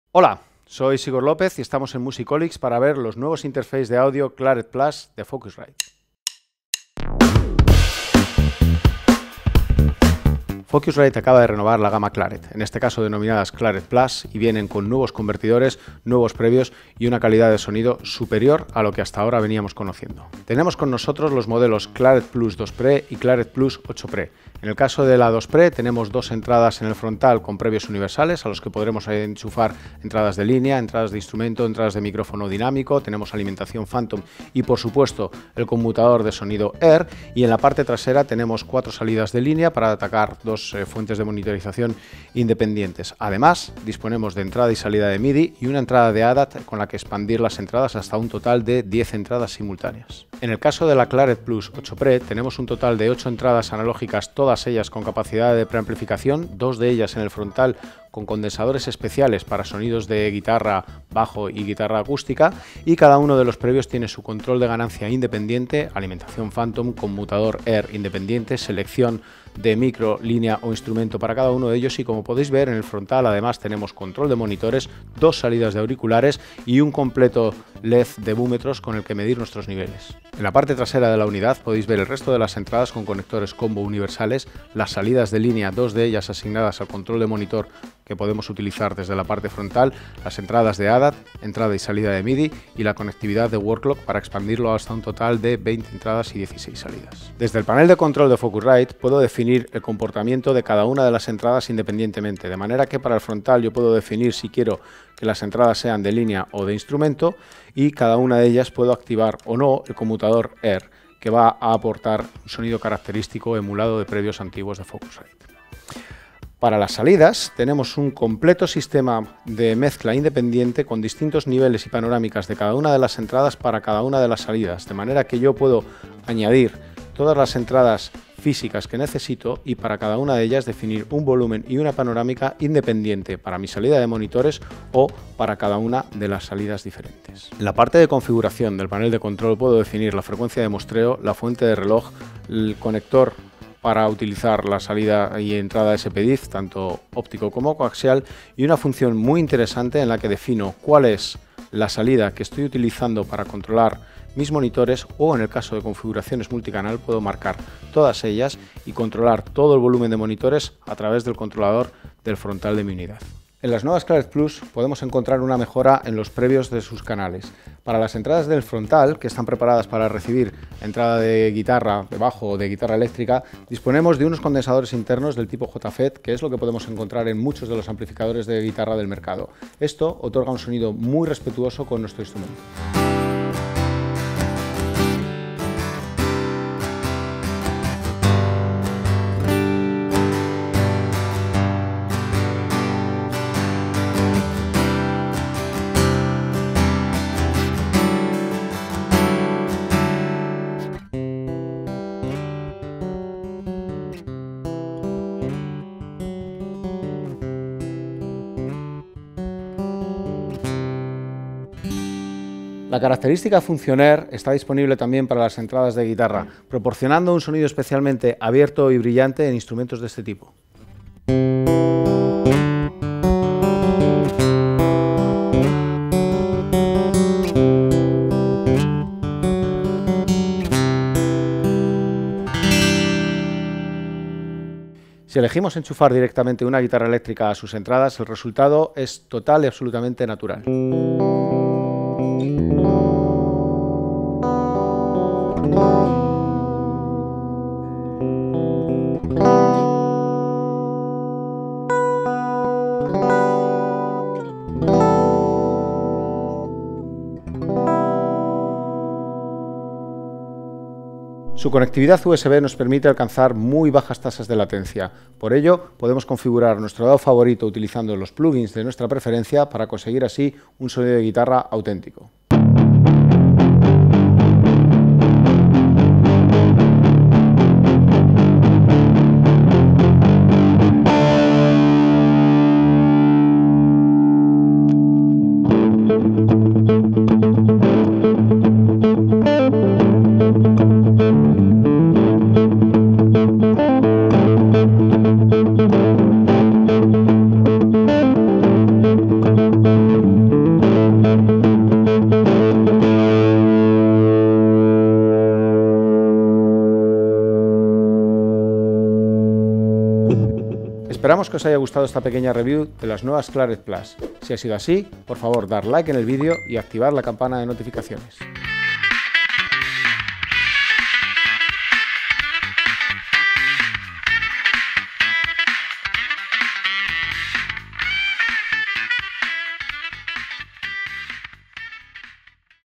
🔴 FOCUSRITE | Clarett+, te presentamos las interfaces USB renovadas con sonido mejorado. En español
Zigor Lopez os presenta los nuevos pre amplificadores Clarett+, totalmente rediseñados desde su predecesor, tienen un rango dinámico muy alto, una baja (muy, muy baja) distorsión y un ruido ultra bajo.

Con ellos conseguirás grabaciones excepcionalmente puras, cristalinas y llenas de riqueza. Un sonido profesional ya sea para grabar voces, guitarras (directas, con micro o las dos cosas a la vez) o baterías.